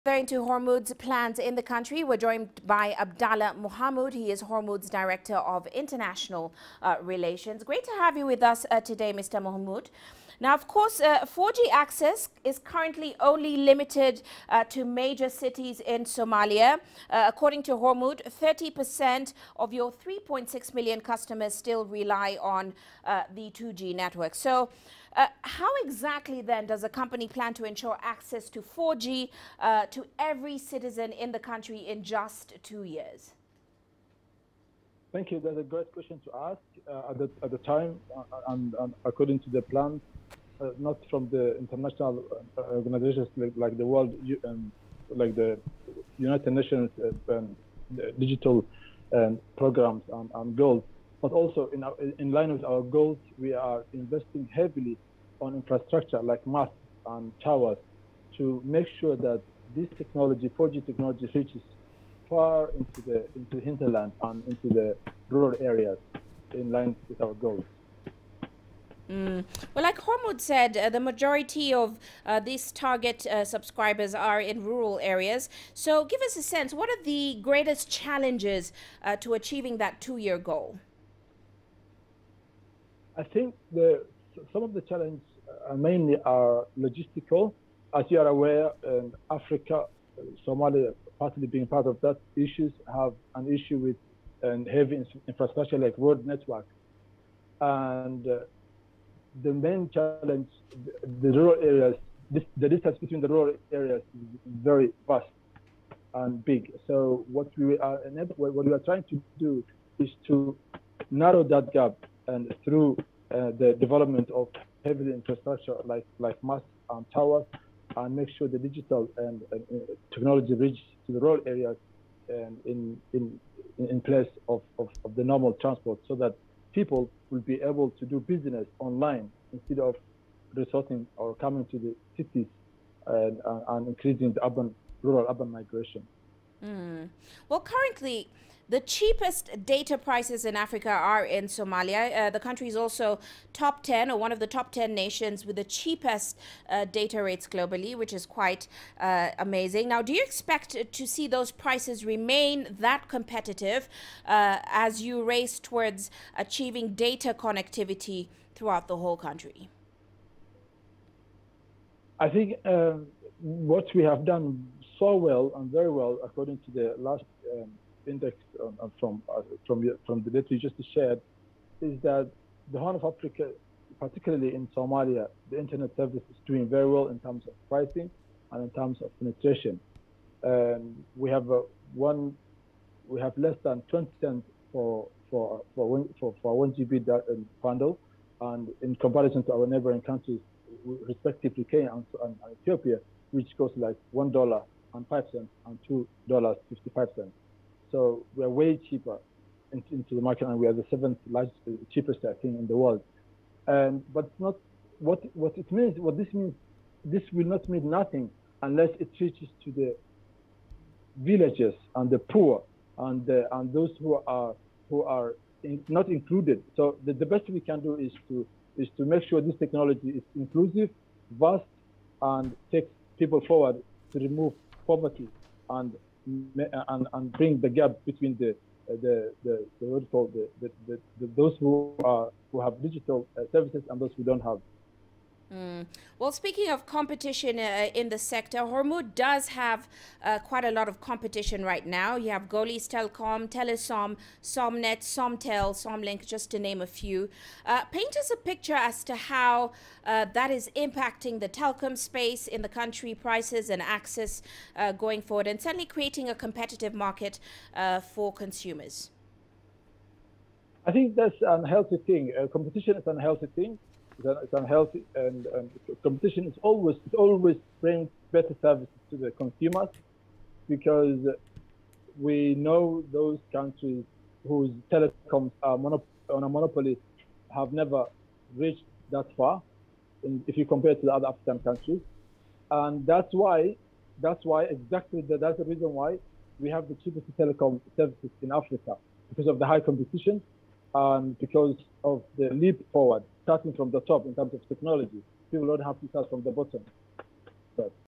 Somalia's 4G expansion plan
Hormuud Telecom is gearing towards extending 4G Networks in the country.